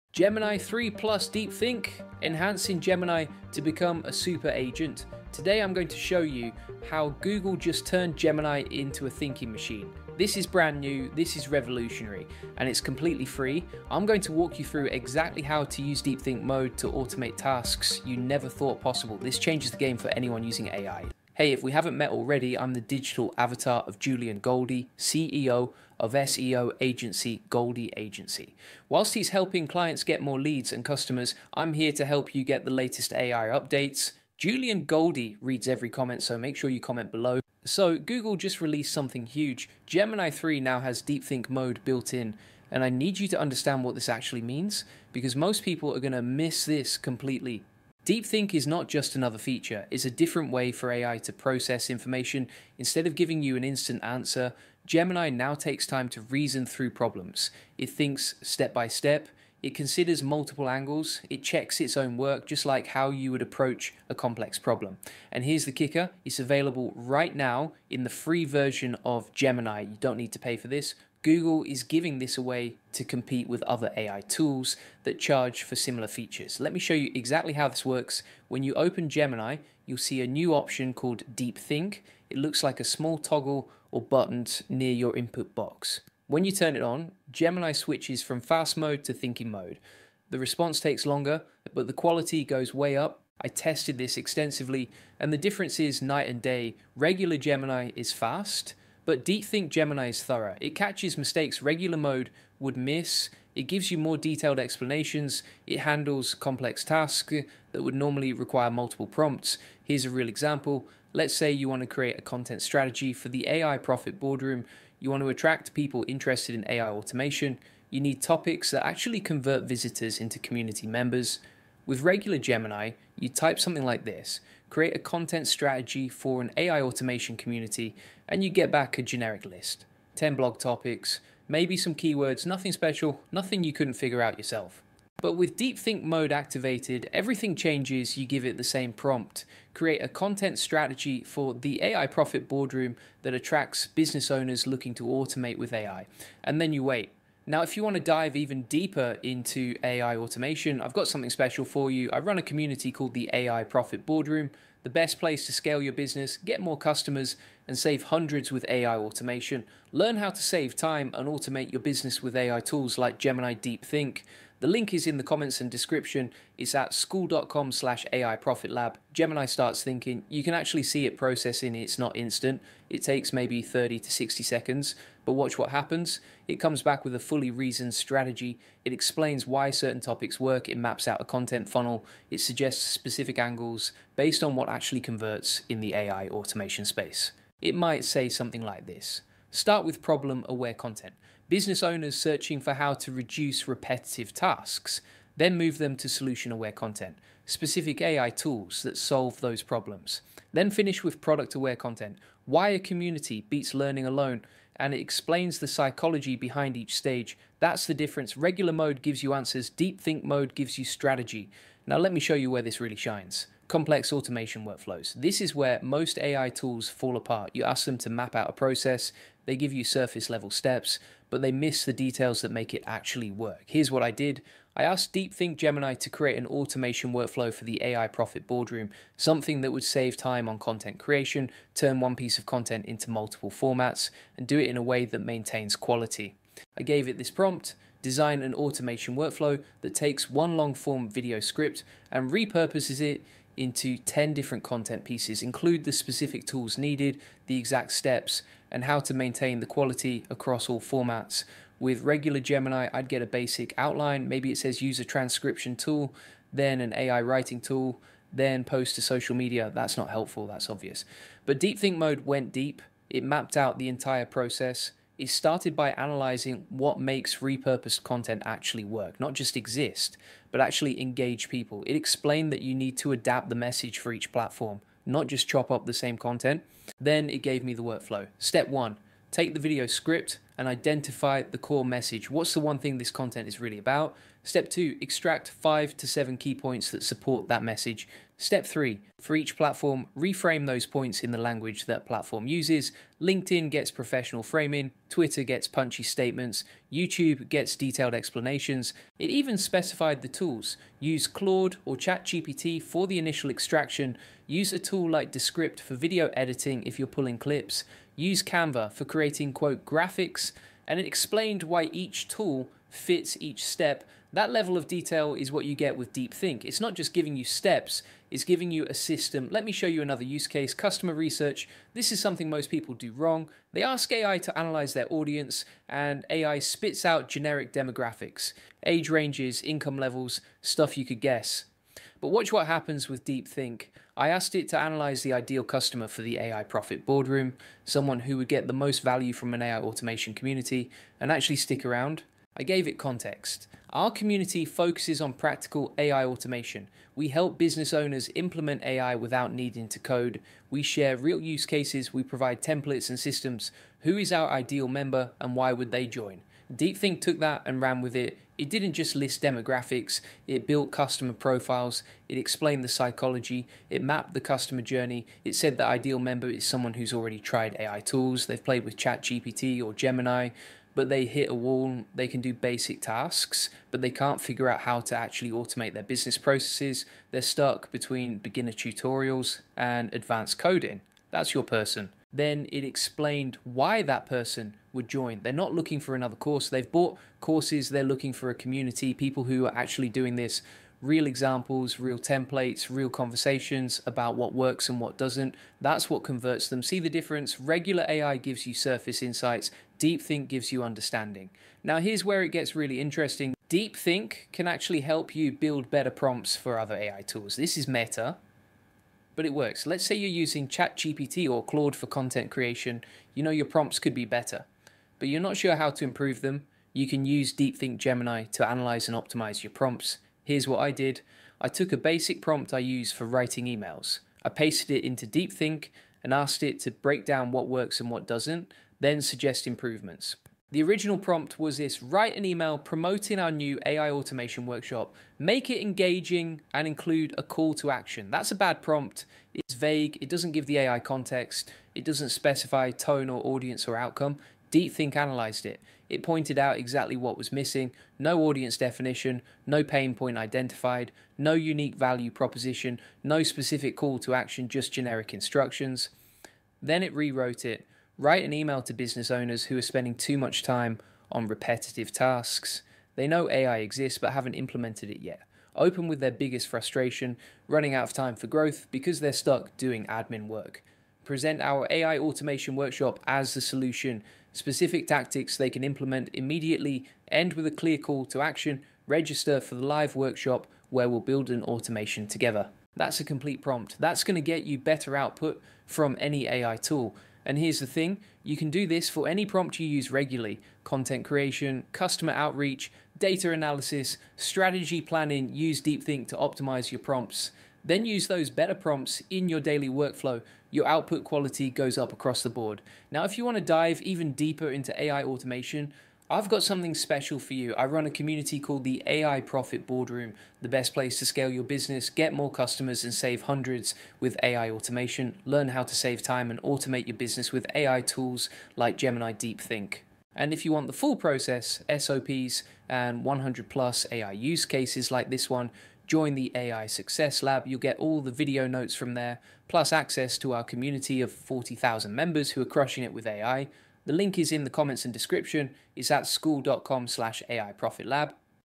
Gemini 3 + Deep Think = Gemini AI Super Agent!
Google Gemini Deep Think: How to Use the New AI Thinking Mode

Discover how Google’s new Gemini Deep Think mode turns AI into a powerful reasoning machine for complex business automation. Learn how to create advanced content strategies, build detailed workflows, and optimize your prompts using this free new feature.

00:00 - 00:00 - Intro
00:38 - What is Gemini Deep Think?
01:19 - How to Activate Deep Think Mode
01:49 - Content Strategy Comparison
03:31 - Building Automation Workflows
05:19 - Advanced Customer Research
06:42 - Optimizing Prompts with Deep Think
08:23 - Scaling with AI Profit Boardroom